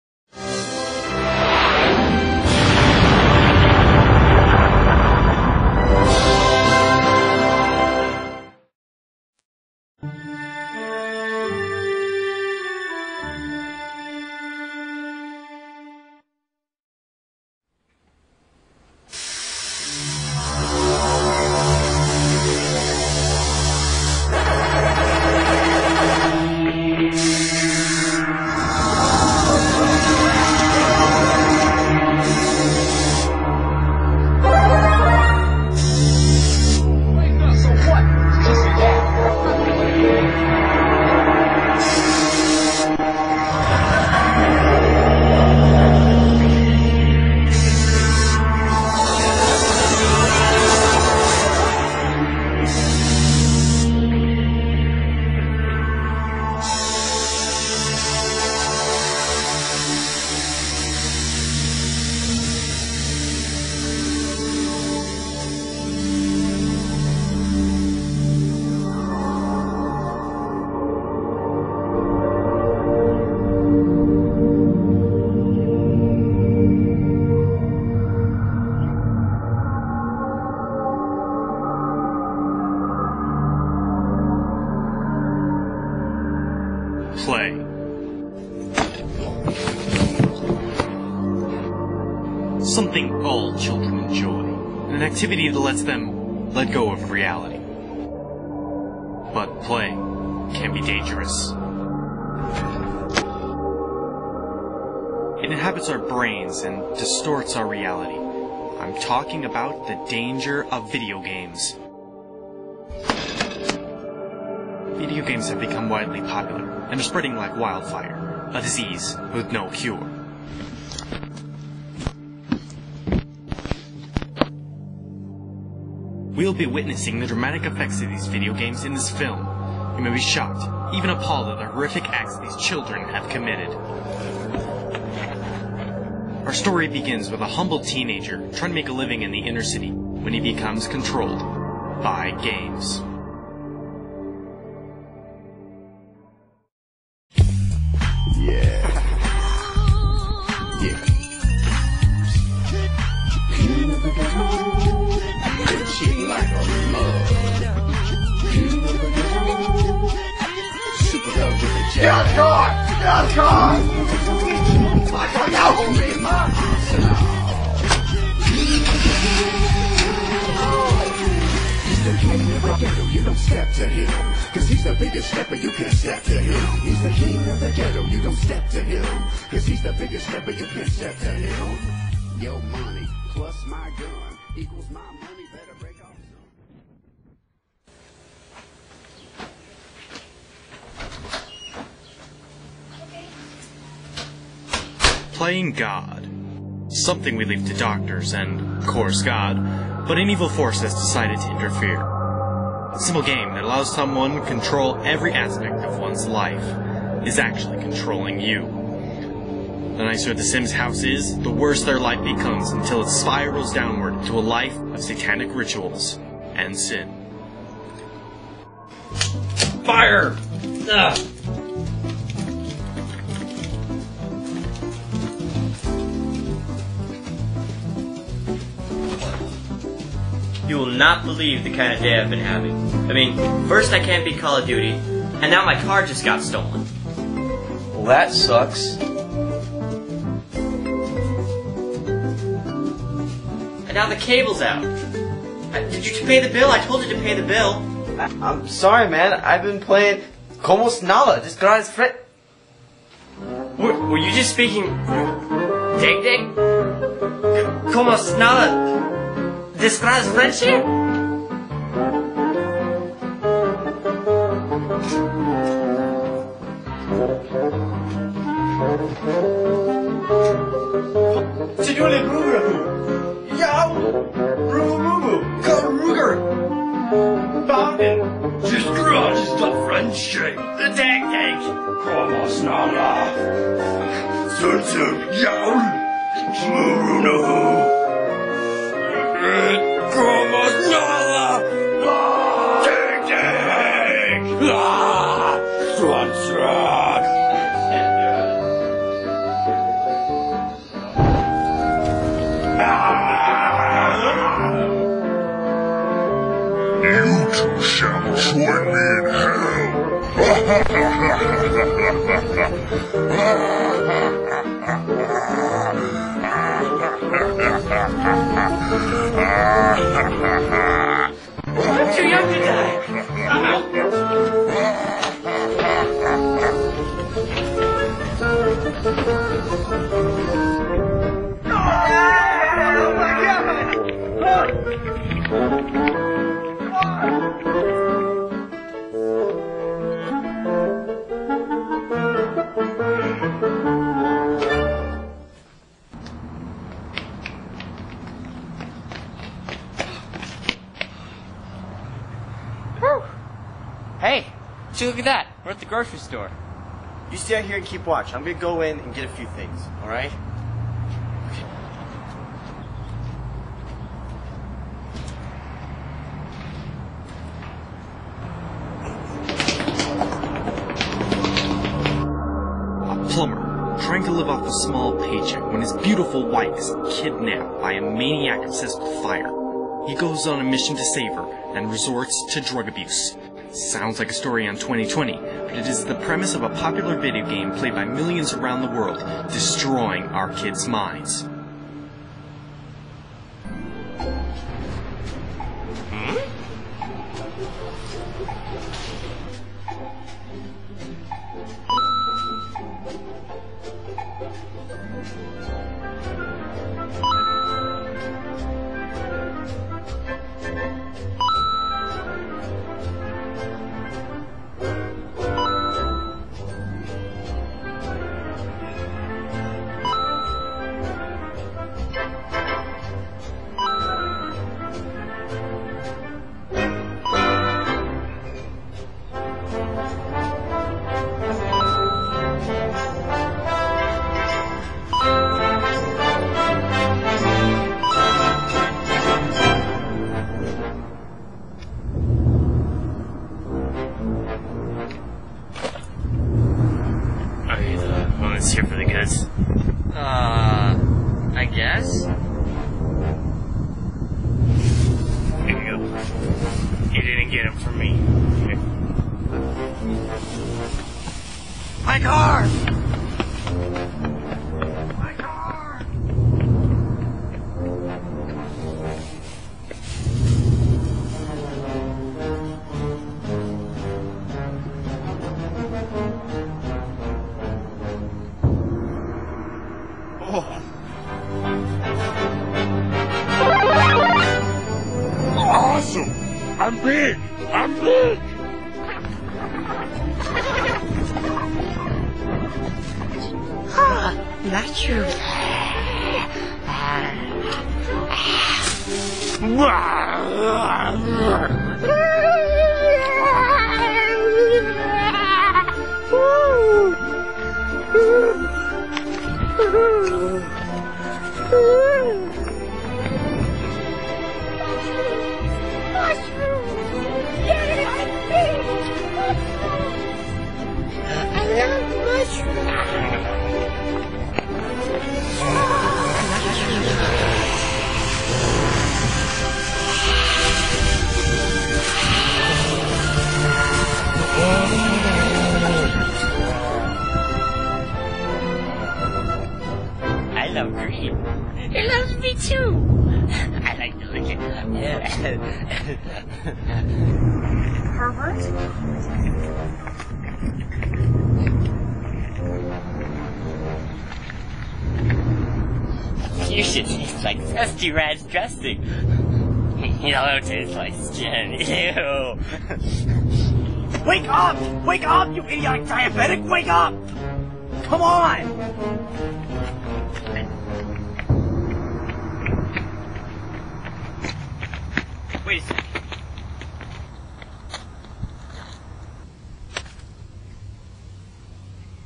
The Dangers of Video Games [Part 1 of 2]
The average American plays 13 hours of video games a week, blissfully unaware of the dangers.

If you liked this video please check out the following sites.